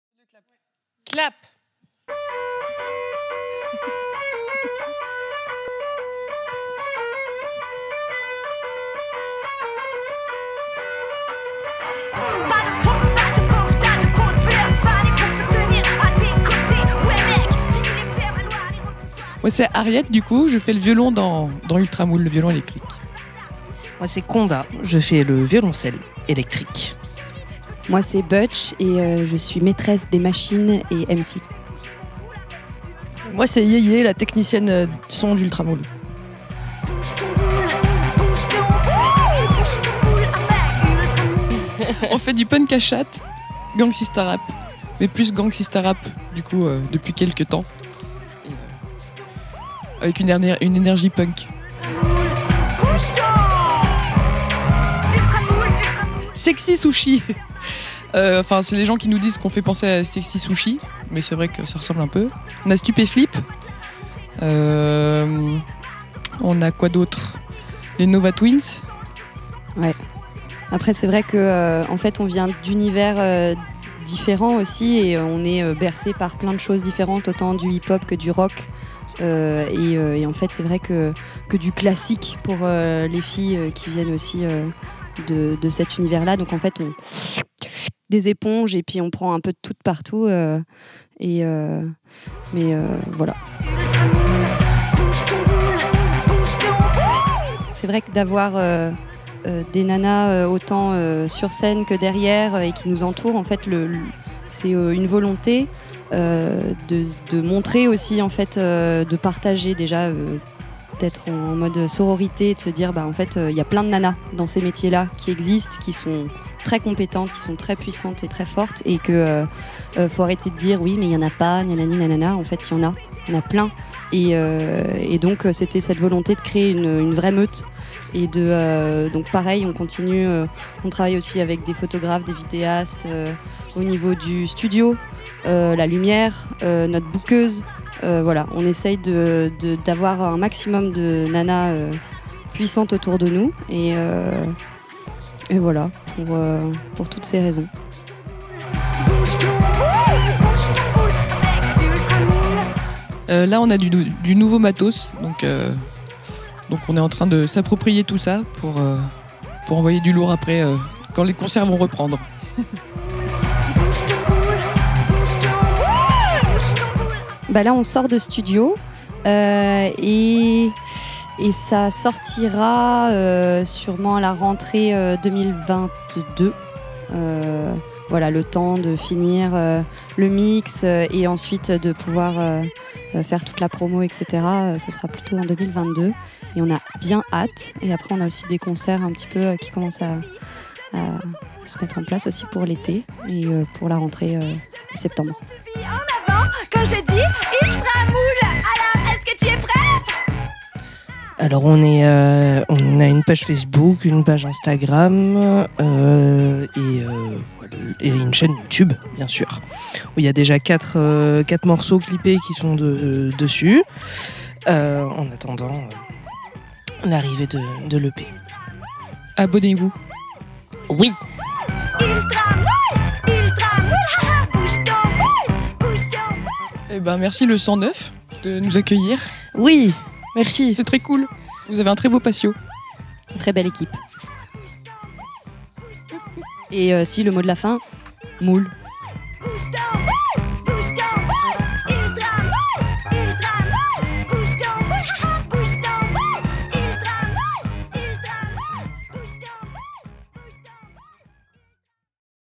Interview UltraMoule
Rencontre avec l'équipe lyonnaise UltraMoule, accueillie à Montluçon lors des concerts en plein air organisés au Guingois à l'été 2020, et revenue début mai 2021 en résidence de 3 jours à l'Embarcadère pour s'approprier leurs nouveaux instruments tout droit sortis des cartons d’emballage !